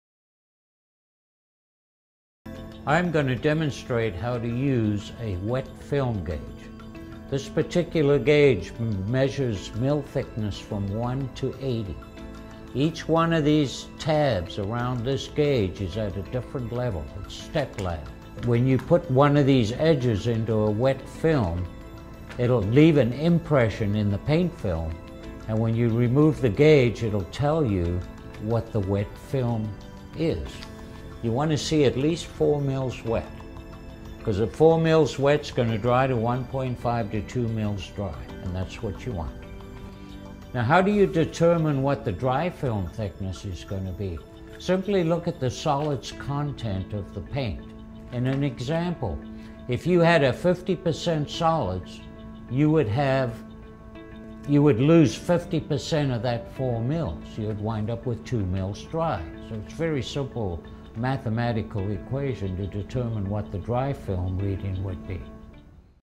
Using Wet Film Gauge | Matthews Paint
Learn how to use a wet film gauge, to determine what the dry film thickness will be.